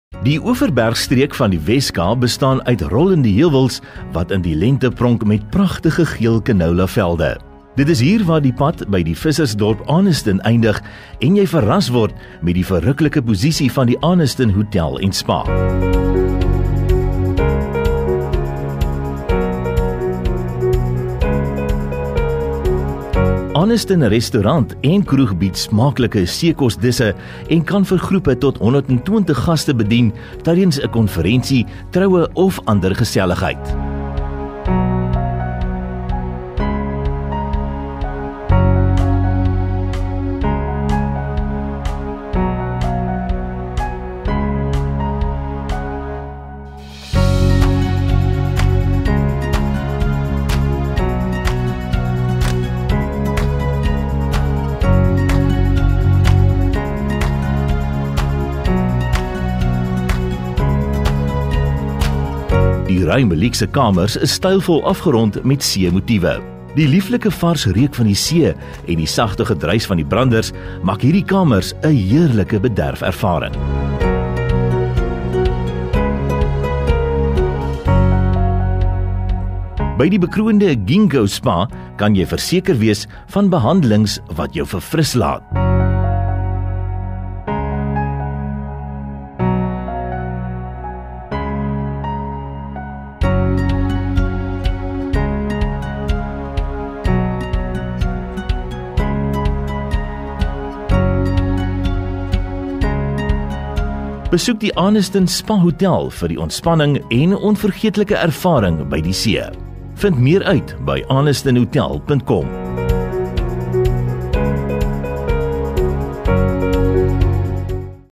kykNET featured the Arniston Spa Hotel
Thank you @kyknet for capturing the luxurious 4-star Arniston Spa Hotel, set in one of the Cape's most exquisite locations. We are surrounded by pristine beaches and nature reserves and are internationally known as one of the world’s great getaways. Our hotel also includes a wonderful relaxation spa.